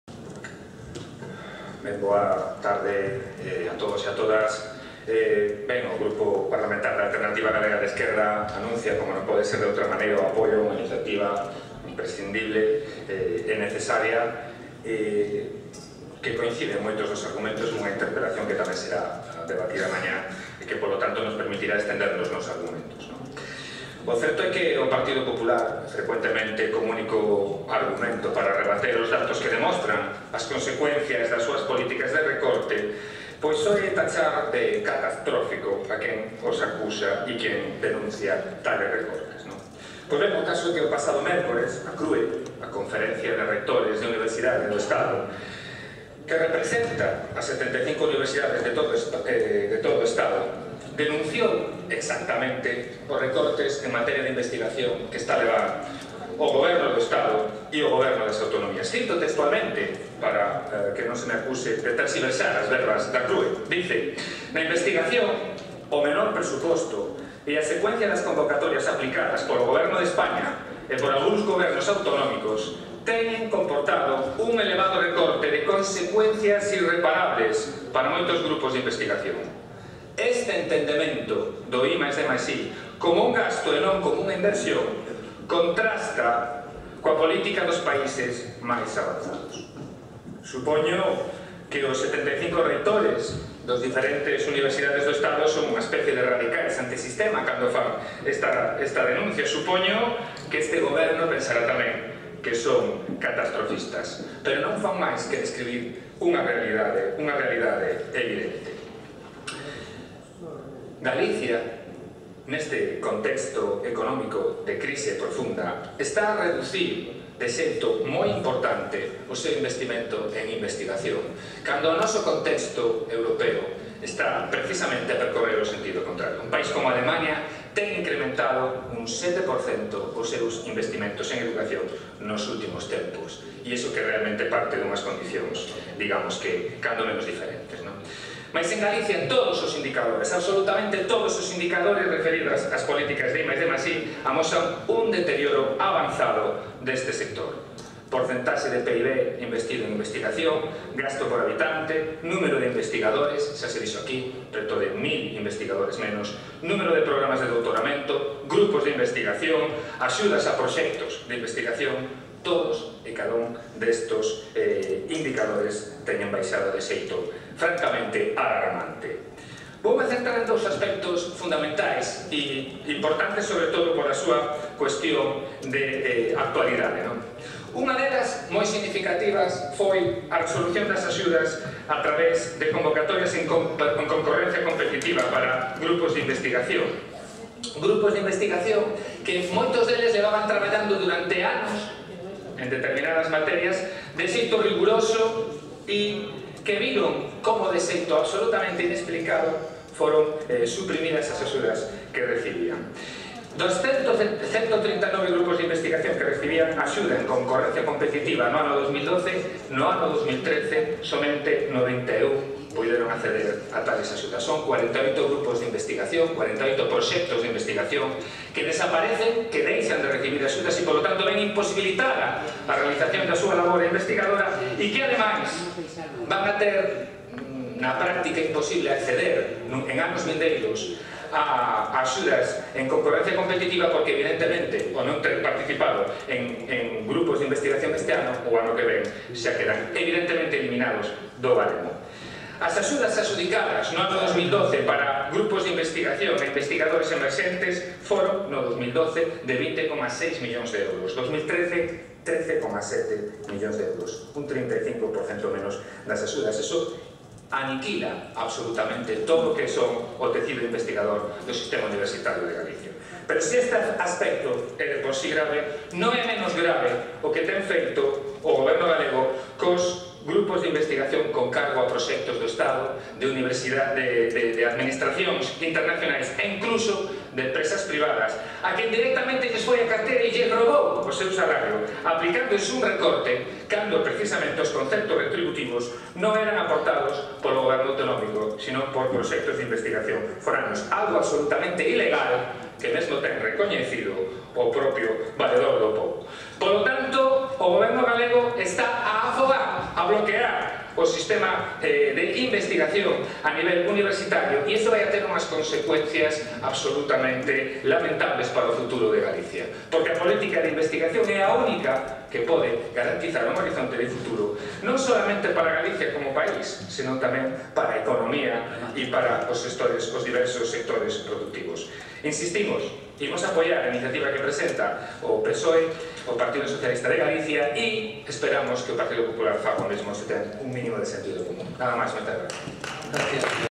Ramón Vázquez denuncia o importante descenso no investimento en I+D+I
O deputado de AGE, Ramón Vázquez, denunciou o importante descenso en investimento en I+D+I por parte do goberno galego e do goberno do Estado que deixa sen futuro ao noso país. Unha liña totalmente oposta a que toman outros países europeos como Alemania onde o investimento en investigación medrou ata un 7 por cento.